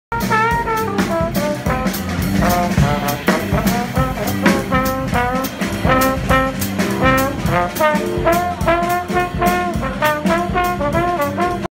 Jazz & Jokes Waukegan Jazz Band
Here's a short clip of the talented Waukegan High School alumni who played jazz music at Bravo's Oct. 2 Jazz & Jokes event. Bravo